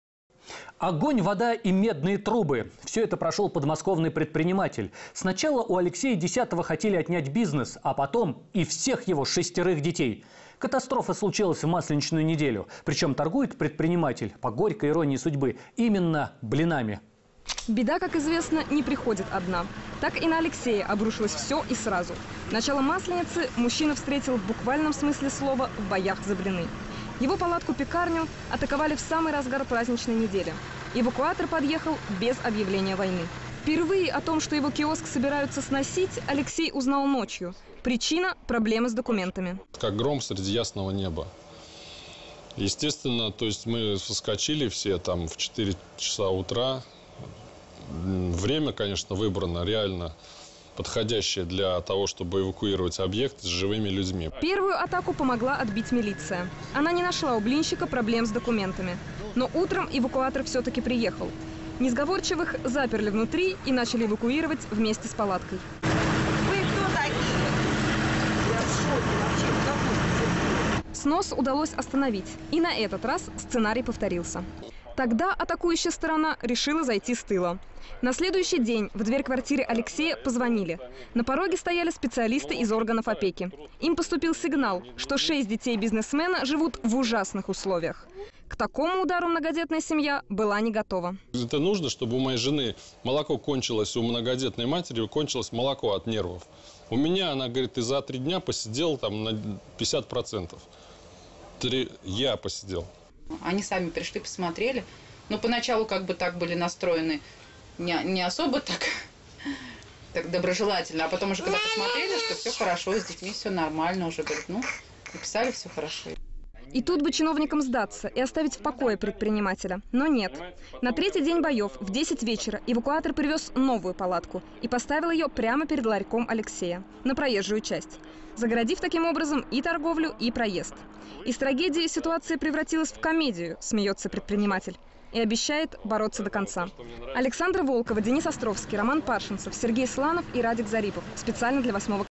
Масленичные баталии
Огонь, вода и медные трубы. Всё это прошел подмосковный предприниматель. Сначала у Алексея Десятова хотели отнять бизнес, а потом и всех его... шестерых детей! Катастрофа случилась на Масленичной неделе. Причем -- по иронии судьбы - торгует предприниматель именно блинами!